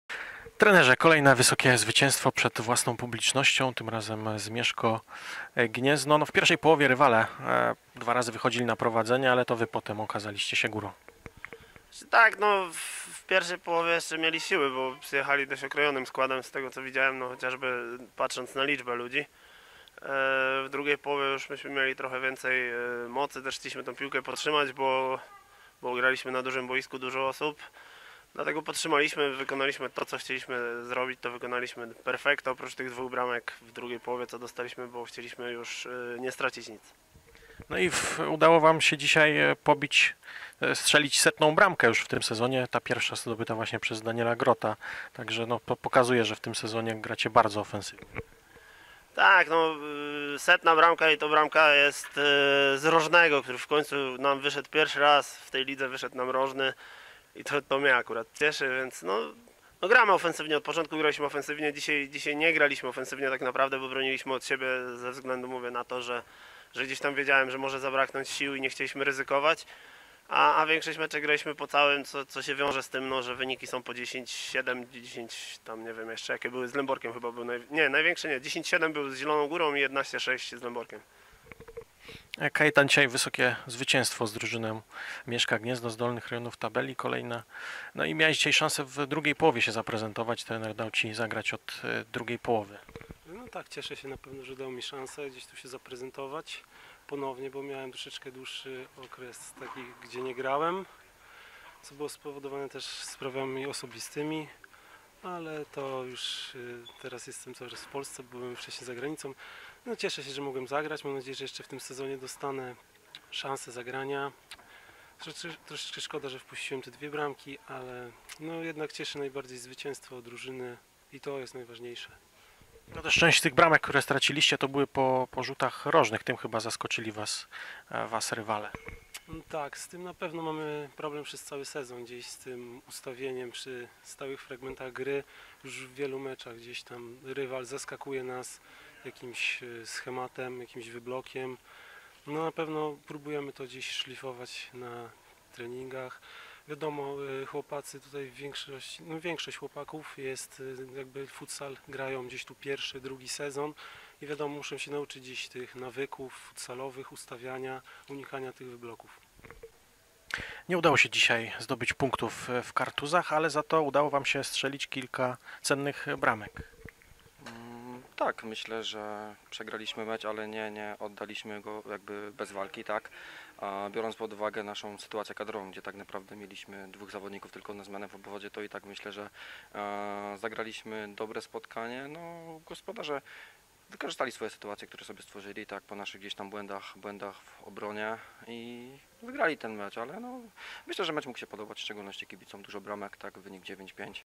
FC Kartuzy - Mieszko Gniezno / Wypowiedzi / 1 Polska Liga Futsalu [08.04.2018]
Zobacz bramki z meczu 1 Polskiej Ligi Futsalu: FC Kartuzy - Mieszko Gniezno z KP Sport